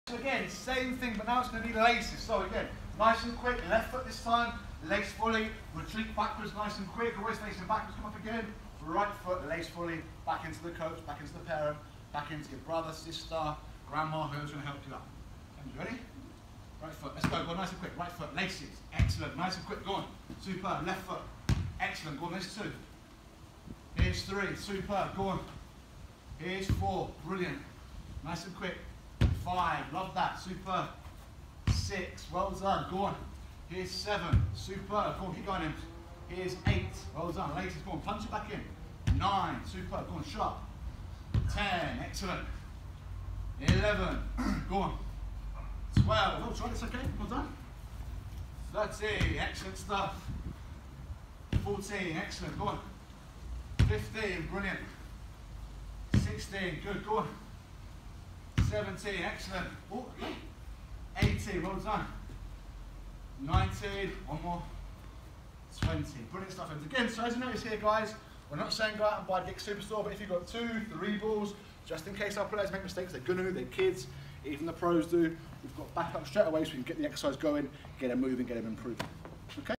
SOUND ON! 🔊 Laces Full Volley ⚽️
✅ Both Feet - laces full volleys
✅ Triangle set up, server ready with ball in hands
✅ Top of the triangle is the start point for the player
✅ Player approaches left side of the triangle and completes a left foot, laces full volley
✅ Player then quickly retreats backwards around the start point cone and forward to the right side
✅ Once at the right side, the player completes a right side, laces full volley and then retreats backwards around the start cone and approaches the left side - forward
✅ Repeat 10 times on each foot
✅ Don’t let the ball bounce before you complete technique